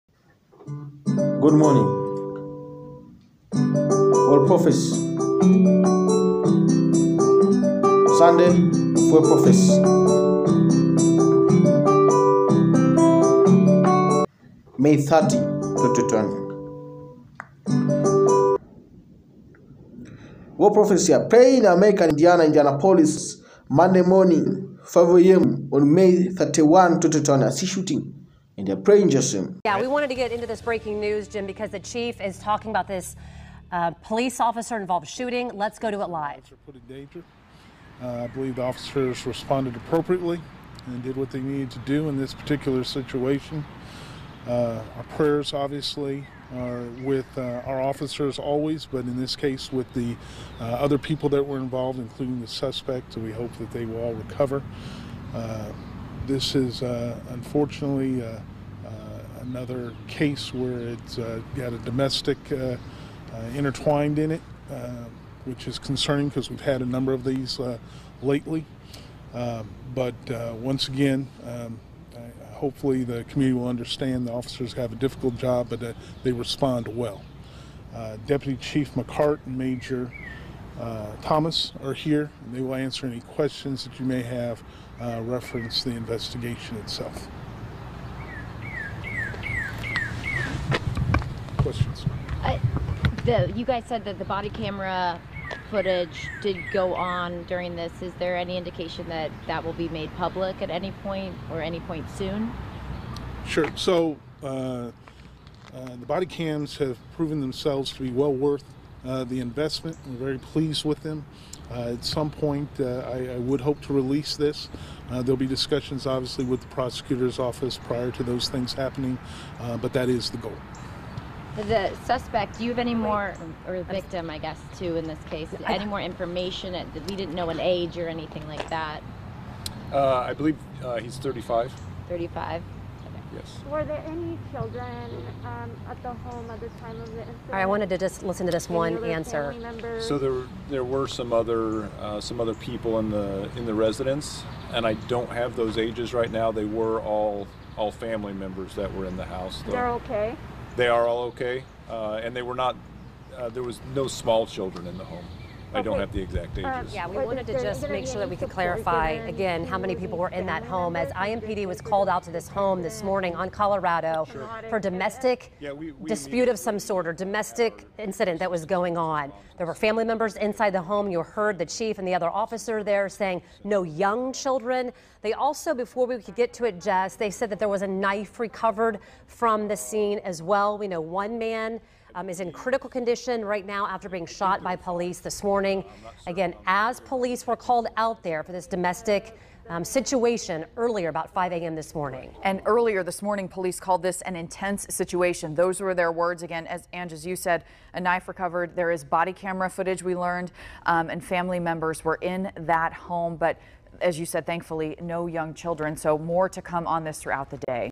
May 31, 2021 Indianapolis Police officer Involved-Shooting in the city of East Side Prophecy
Sunday Of World Prophecy May 30,2021
 World Prophecy .🇺🇸
I Praying in America Indiana Indianapolis Monday Morning 5a.m of May 31,2021 i see Shooting and I Praying in Jesus Name.

May 31,2021.
Indianapolis Police officer Involved-Shooting in the city of East Side Monday Morning Around 5a.m May 31,2021.

GoodMorning.

good.morningtv
GoodMorning.TV

Good Morning. TV